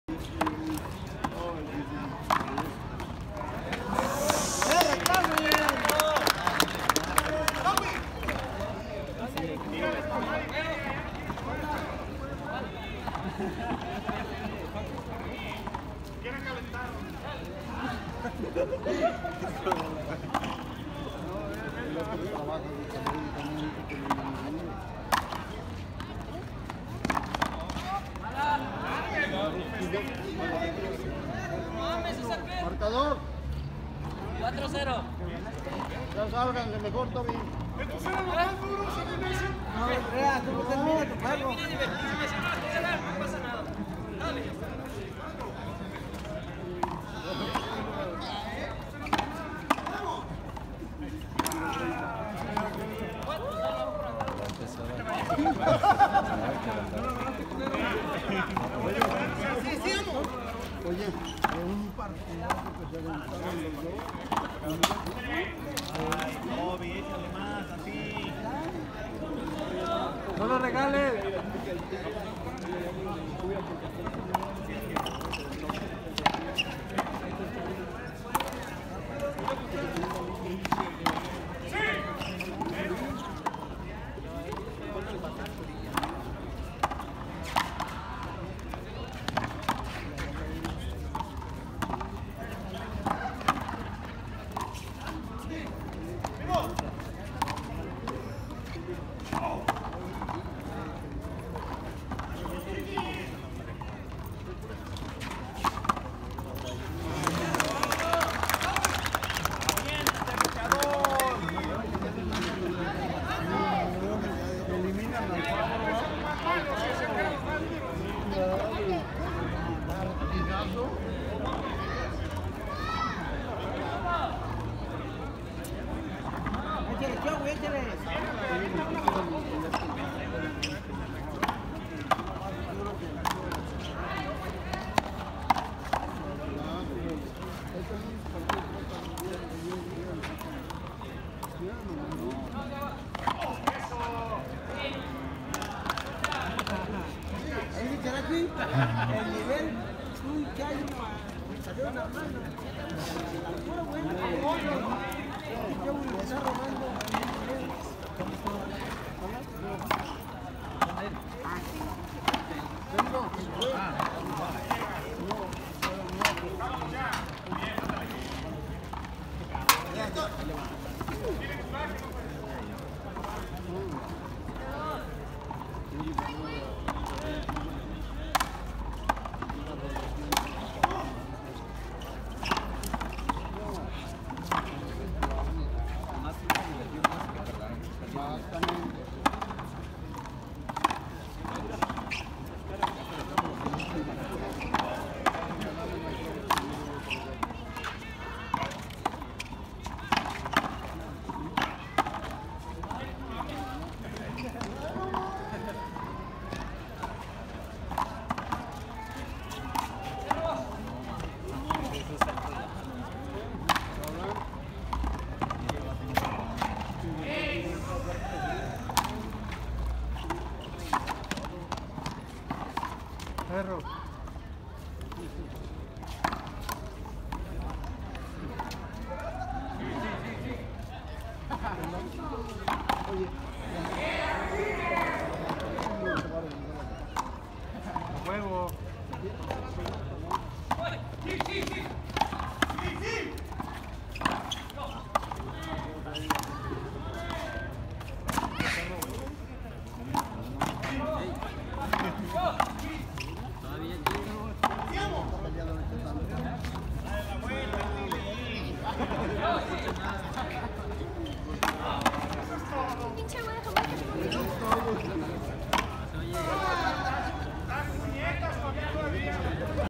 Torneo de Frontón en Tabachines (parte 161)
Frontón a Mano Jalisco 15 de septiembre en la Unidad Deportiva Tabachines.